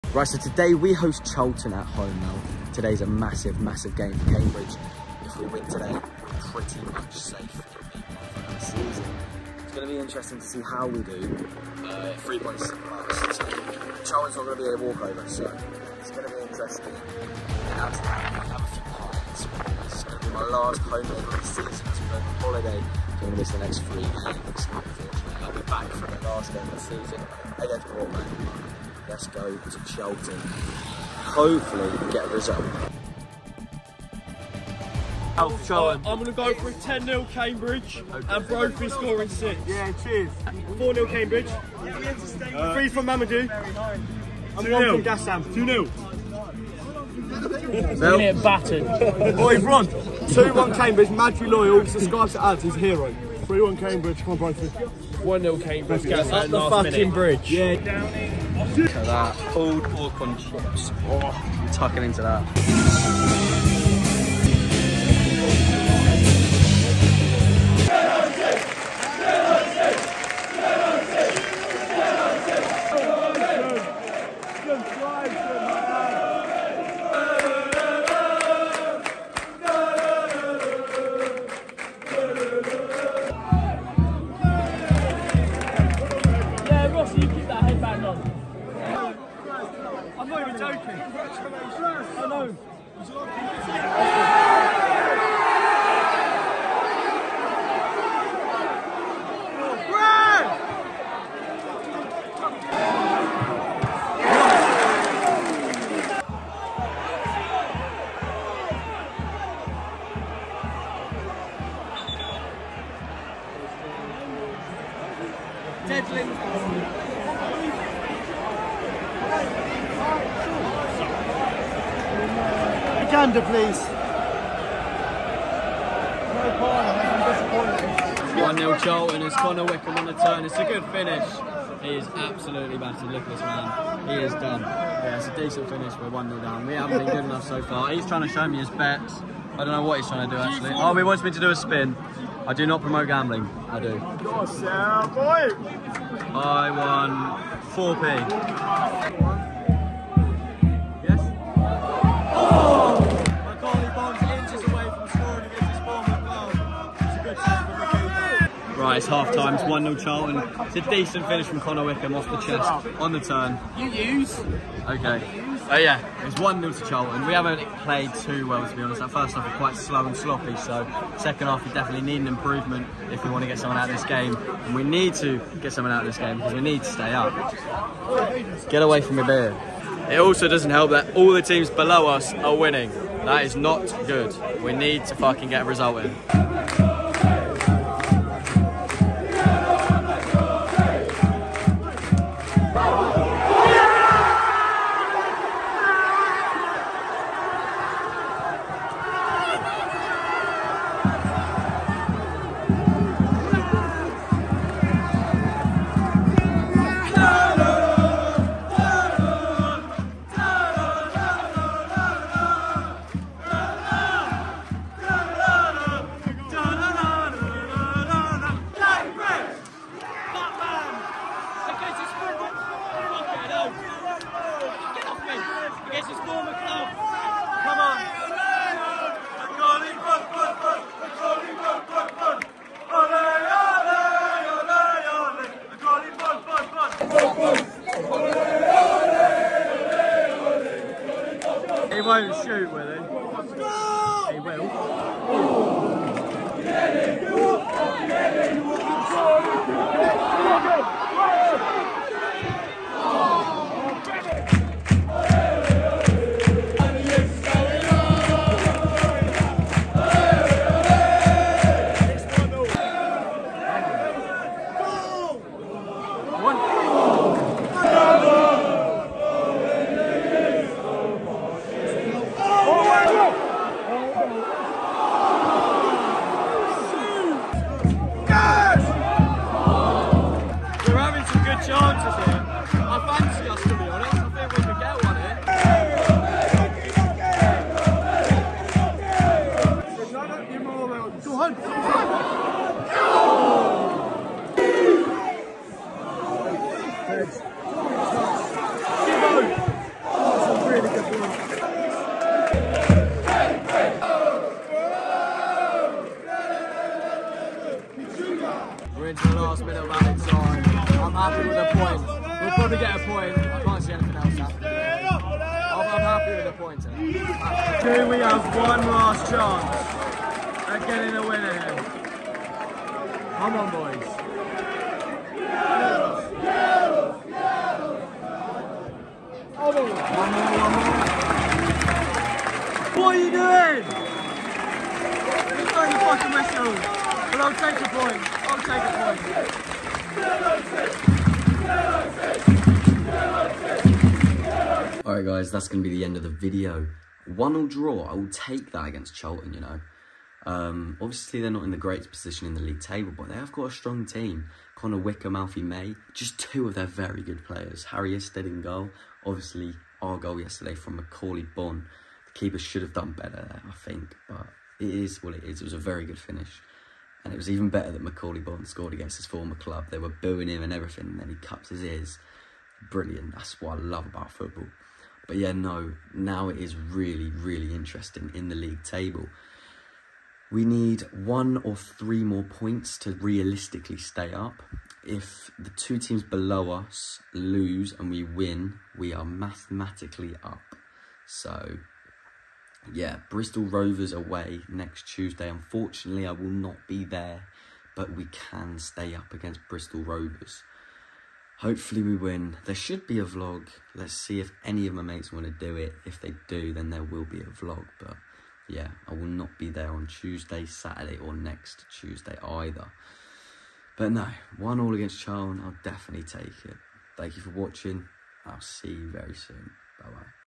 CAMBRIDGE vs CHARLTON | 1-1 | HOME LIMBS & SOLD OUT AWAY END
JUST 3 MORE POINTS AND WE’RE SAFE!

Enjoy this Charlton vlog as we got a good draw against a strong side thanks to Macauley Bonne! It was a lovely touch, turn, shot. Very good finish for his first goal for the club and a very important goal too!

We face Bristol Rovers on Tuesday in which is the biggest game of the season as we could stay up! Hopefully thag happens and we’re not waiting till the final day to stay up, my nerves cannot take that again.

I will unfortunately be away for 3/4 last games as i’m on holiday however, i will be back for Port Vale away on the final day of the season!

UP THE U U'S